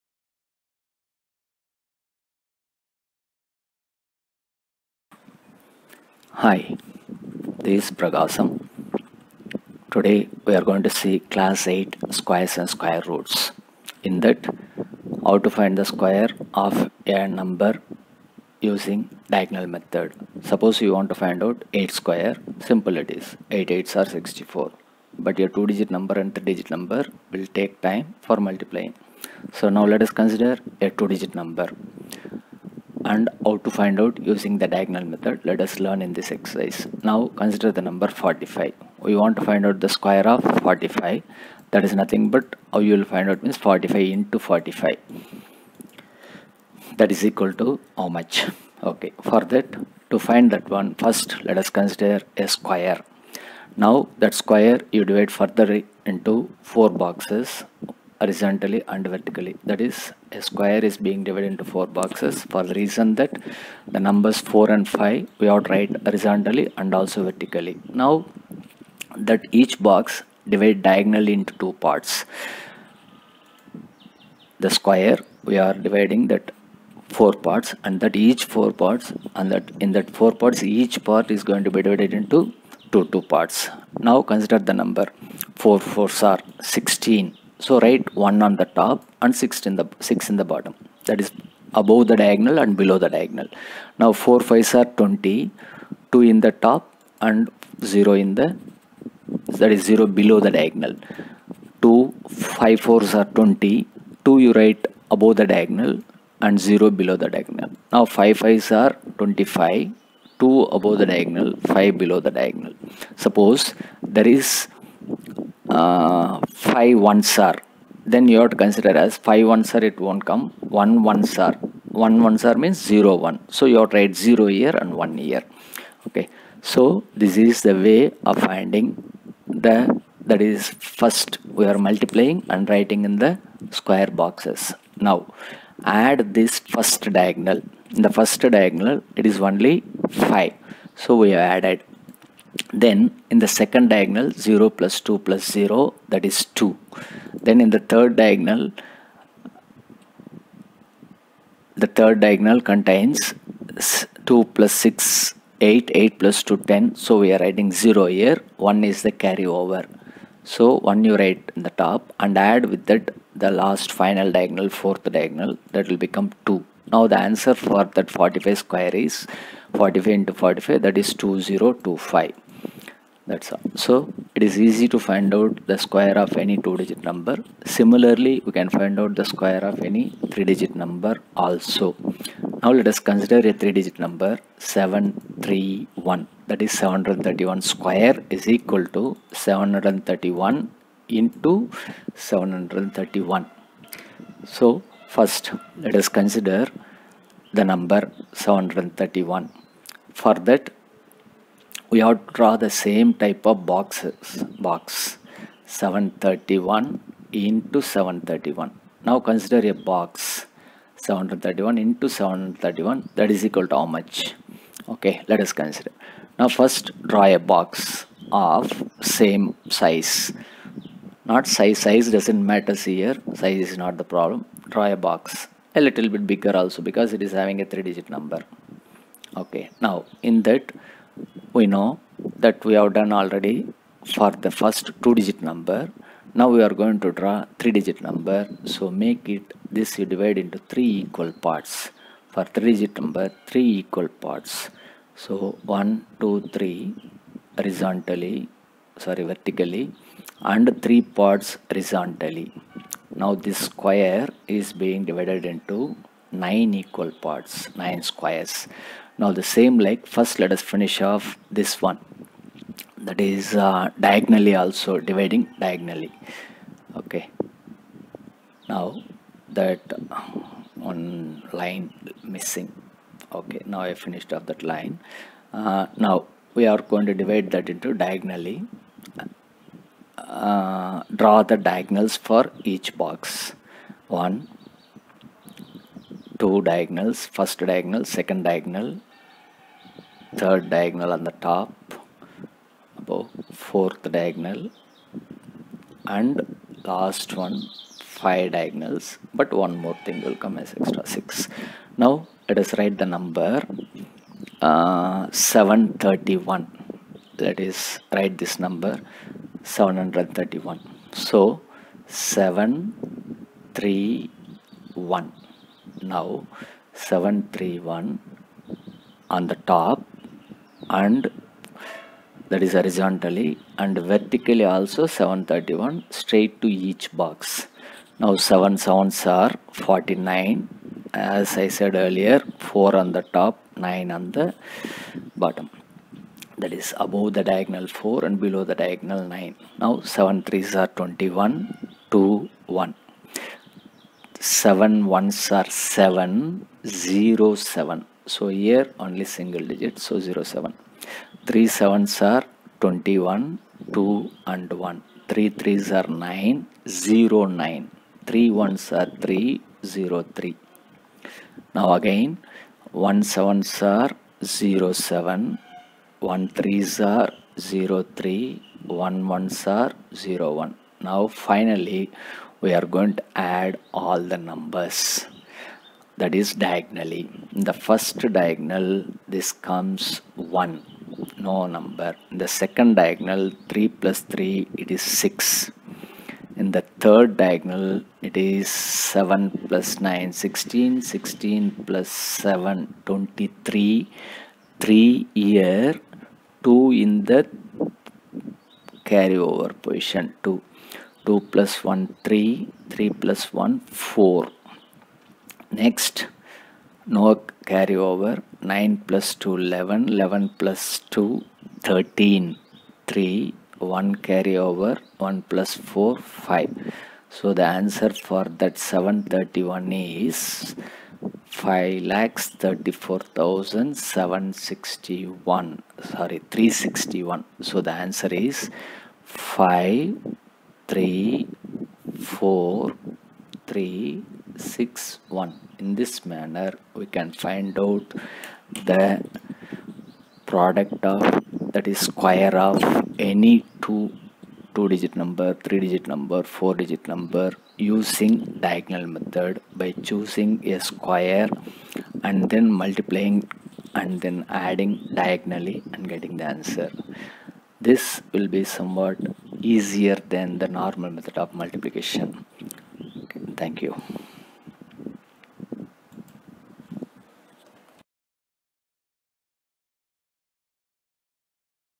Finding the Square by diagonal method
Steps of finding out square by diagonal method.
 1) Form a square and divide it into sub-squares based on the number of digits, row and column wise. Then draw the diagonals of the smaller squares.
2) Multiply each digit on the left of the square with each digit on the top of the column one by one.
3) If the product has a single digit number, write it below the diagonal. If it has two digit number, write the tens digit in upper part of the diagonal and units digit in the lower part of the diagonal.
4) Add all the digits diagonally starting from the lowest diagonal.
5) Underline all the units' digits and carry over the other digit to the next and add the carry to the next number. 6) After completing this write down all the underlined digits. This number will be the square of the given number.